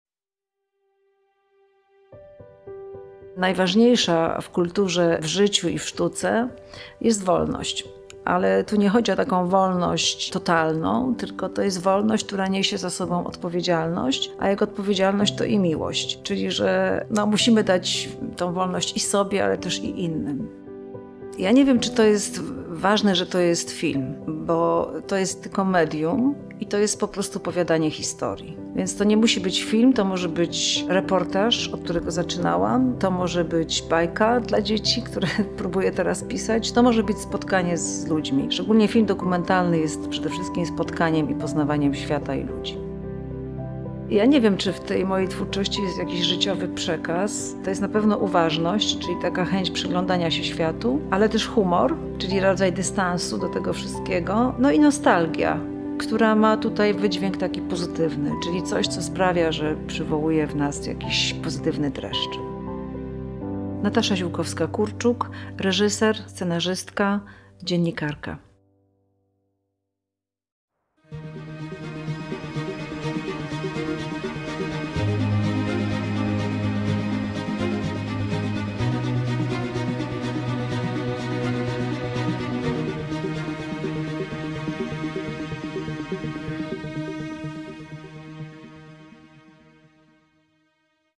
Natasza Ziółkowska-Kurczuk - rekomendacja do Nagrody Miasta Lublin za Całokształt Działalności
Natasza Ziółkowska-Kurczuk – profesor sztuk filmowych i teatralnych w Katedrze Dziennikarstwa UMCS, reżyserka, scenarzystka, dziennikarka, absolwentka dziennikarstwa i kulturoznawstwa na UAM oraz reżyserii w PWSFTiT w Łodzi. Autorka reportaży społeczno-kulturalnych publikowanych we „Wprost” i Tygodniku Pracy Twórczej „Radar”. Jej dokonania artystyczne to przede wszystkim filmy dokumentalne i widowiska telewizyjne, których bohaterem jest Lublin albo osoby związane z Lublinem. Filmy Ziółkowskiej były wielokrotnie nagradzane i pokazywane na specjalnych projekcjach i festiwalach w Polsce i za granicą. Były i są emitowane w stacjach telewizyjnych. Realizuje program kulturalny Afisz. Jest autorką reportaży i relacji z ważnych lubelskich festiwali. Laureatka nagród i stypendiów. Autorka widowisk baletowych i spektakli. Autorka publikacji i artykułów naukowych, jurorka, instruktorka warsztatów medialnych i filmowych, opiekunka kilkudziesięciu filmów studenckich.